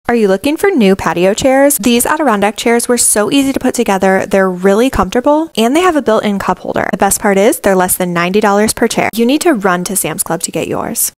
Sam's Club Patio Chairs, Adirondack Chairs, Patio Upgrade
New patio chairs to finish our beautiful patio!